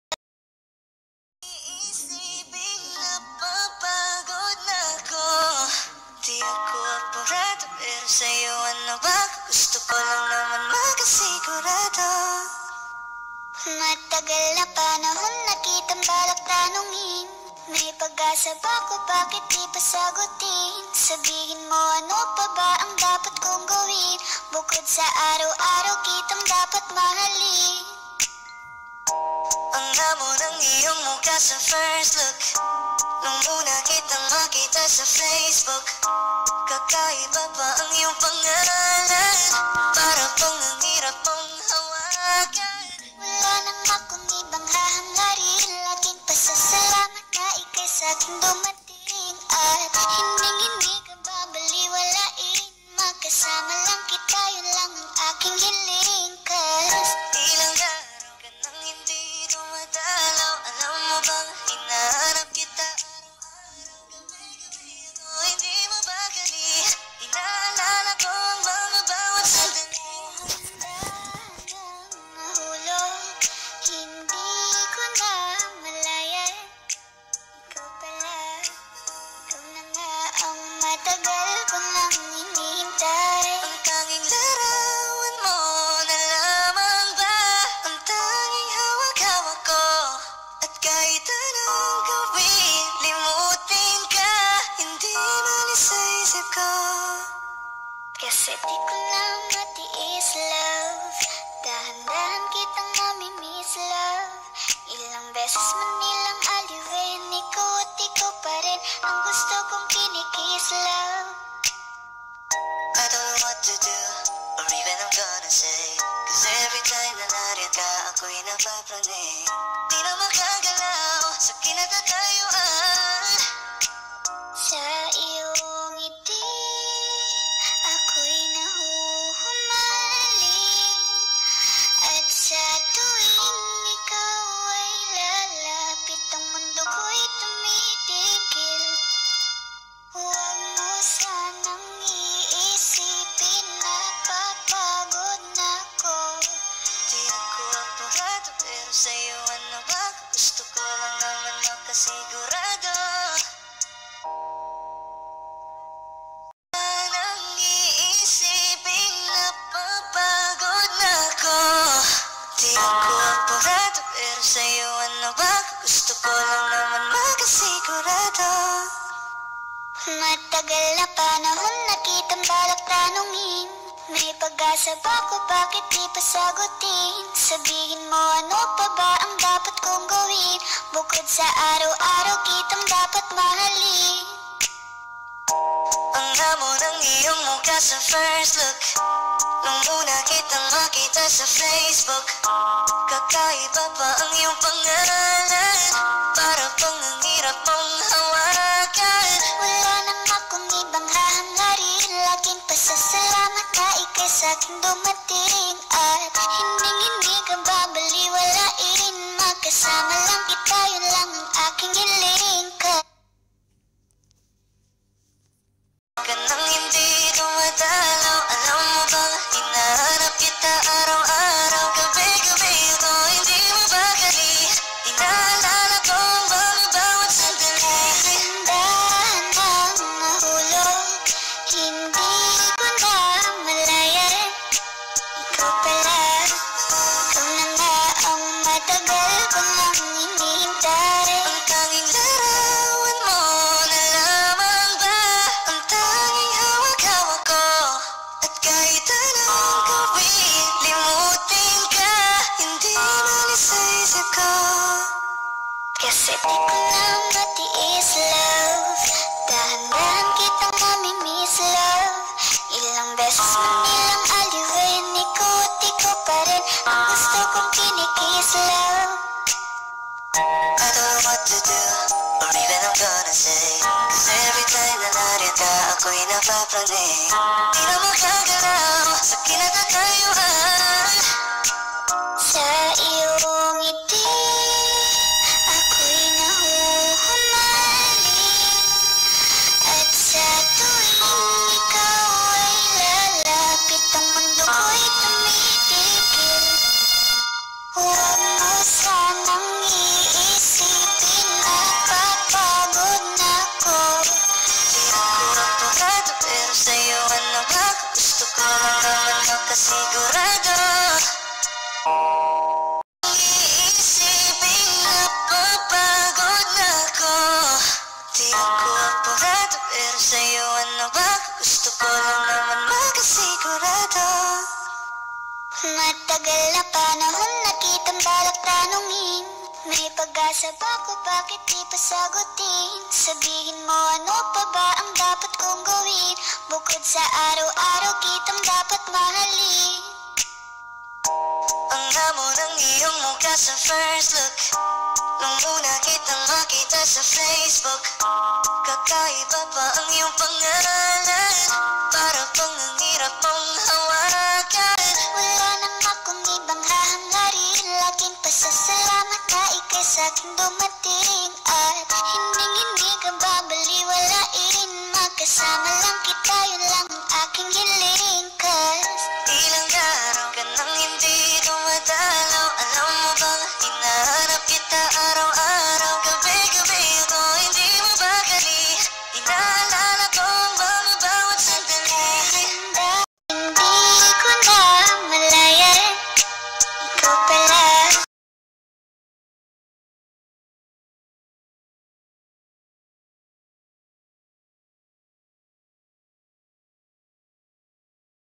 I MEET ANNEMARIAH/NAGUTOM OFW SA BHARAIN MIMO VLOGZ
namasyal nagutom kumain sa karzakan#

Thank you so much guys for watching my video sana bill nyo natafacation para updated po kayo sa next video ko merci